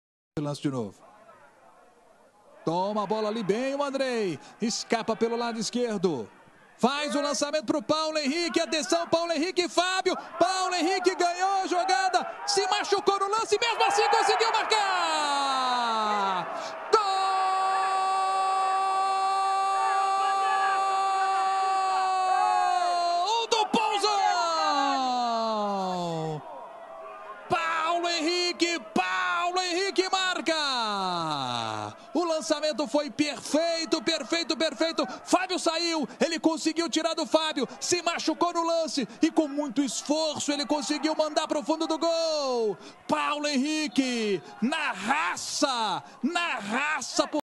SAKAT SAKAT GOLÜ ATTI, MAÇI KAZANDIRDI..PAULO HENRİQUE
Brezilya Mineiro 1 Ligi’nde oynanan Pousa Alegre - Cruzeiro maçına Paulo Henrique damga vurdu. Eski Trabzonspor'lu Henrique gole gittiği sırada sakatlanmasına rağmen pozisyonu devam ettirip golü attı ve attığı bu golle takımı maçı 1-0 kazandı.